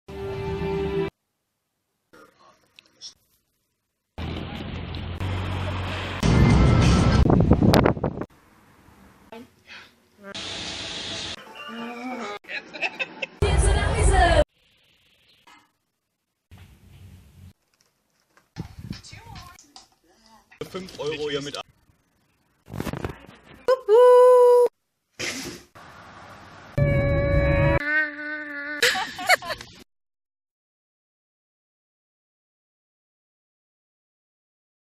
Mein August 2015 - One second everyday
Mithilfe einer App habe ich jeden Tag eine Sekunde Video aufgenommen. Hier seht ihr das Ergebnis. (An zwei Tagen habe ich es vergessen xD)
Die App ist kostenlos und heißt 1SecondEveryday!

Wenn ihr mir eine Freude machen wollt, hinterlasst einen Kommentar mit "Mimblewimble" :D